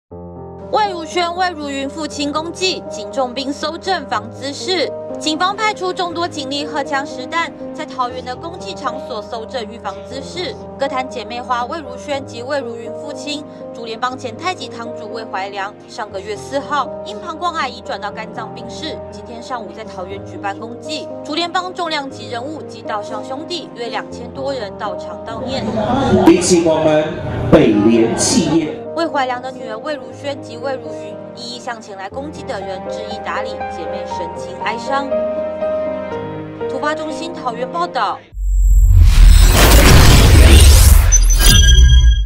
魏如萱、魏如昀父親公祭 警重兵蒐證防滋事 | 台灣蘋果日報
歌壇姊妹花魏如萱及魏如昀父親、竹聯幫前太極堂主魏懷良（60歲），上月4日因膀胱癌移轉到肝臟病逝，今天上午在桃園舉辦公祭，竹聯幫重量級人物及道上兄弟，約2000多人到場悼念，公祭場合裡，魏懷良一對女兒魏如萱及魏如昀，一一向前來公祭的人致意、答禮，姊妹神情哀傷，因公祭場面盛大，警方派出眾多警力蒐證。